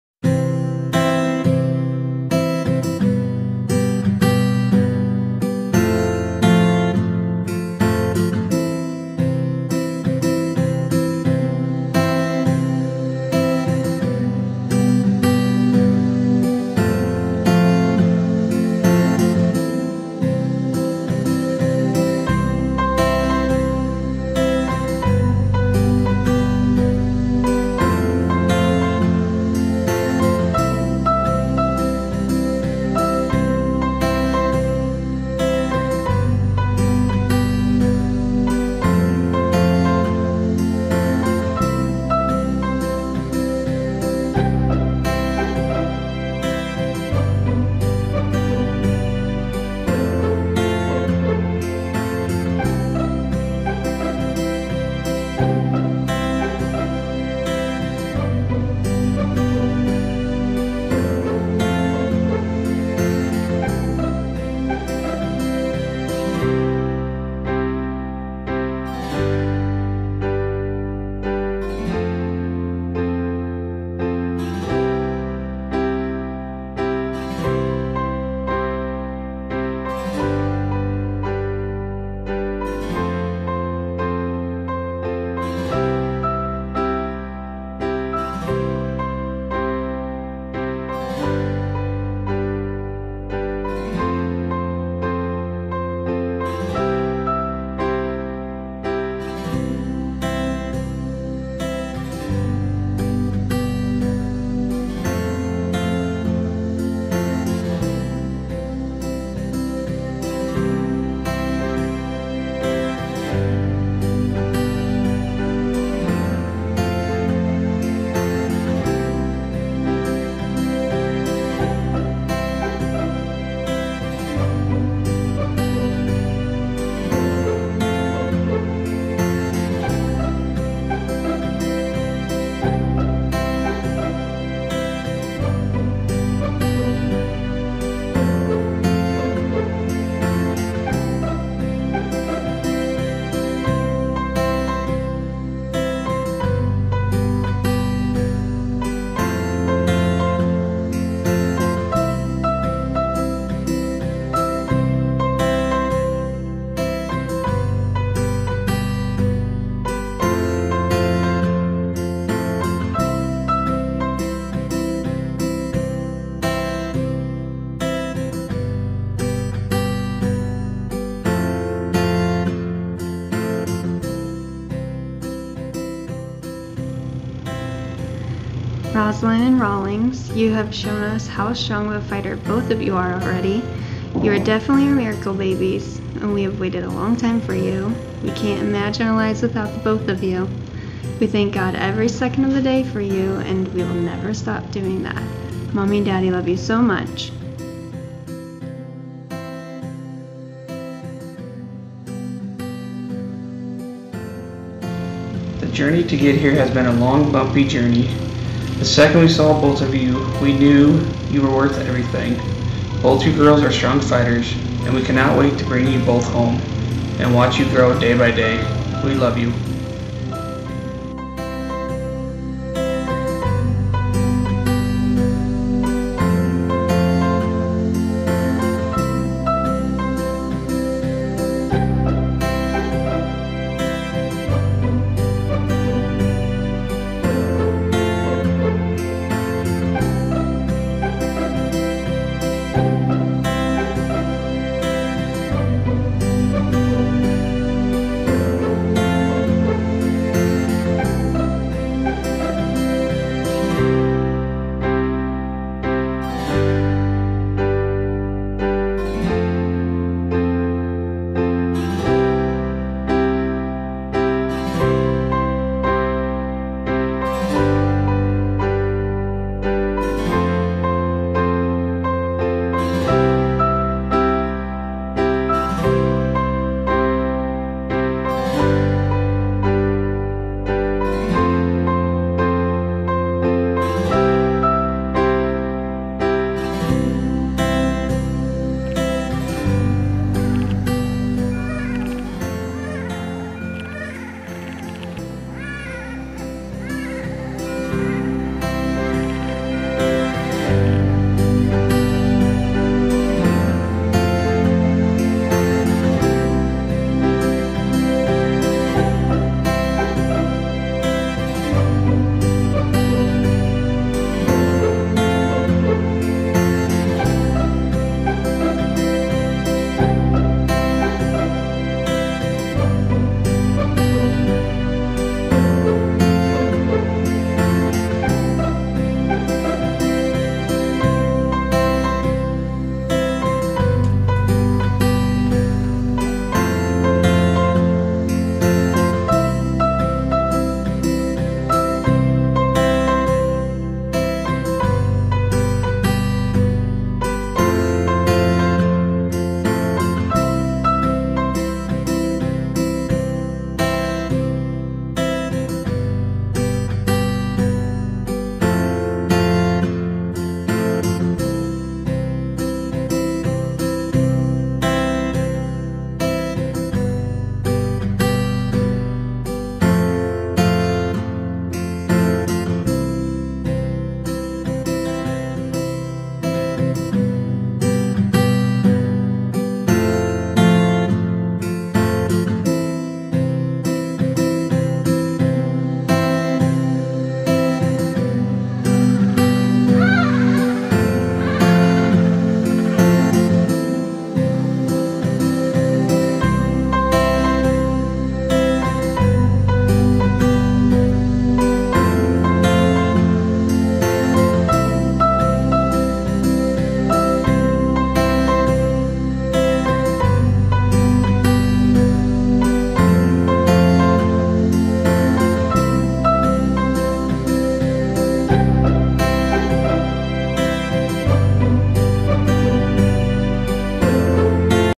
Inspired by Soul Photography-Classen Twin Girls-6th Day at NICU
These two little ladies were born at 29 weeks and 5 days. It was a honor to come to the Women's Methodist Hospital in Omaha NE to create this short documentary film for their family to cherish forever.